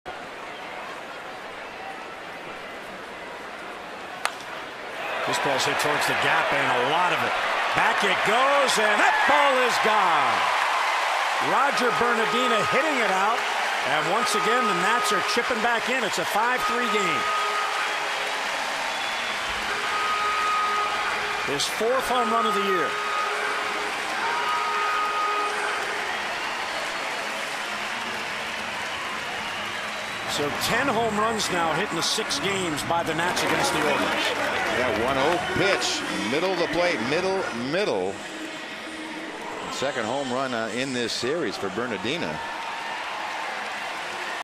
2011/06/19 Bernadina's solo tater
BAL@WSH: Bernadina belts homer to center in the fifth